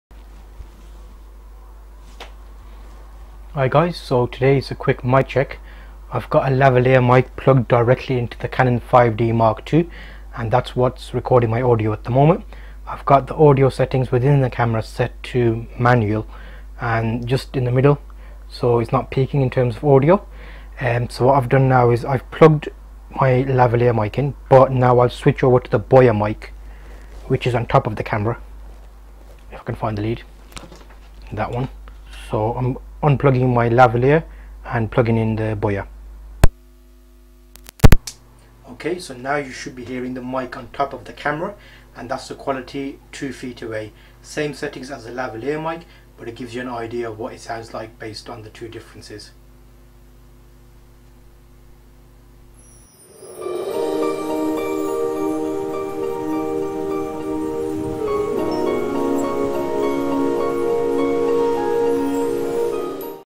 Best budget mic for YouTube video recording
Get a free link to my resource list, which includes everything discussed in this video, Have you caught up on the last episode? Here i plug in a lav mic direct into a 5d mk2 and then switch to the Boya mic on top of the camera. Use headphones you can tell the difference. Best budget mic for YouTube video recording

Cheap Action Camera - Goodmans 1080p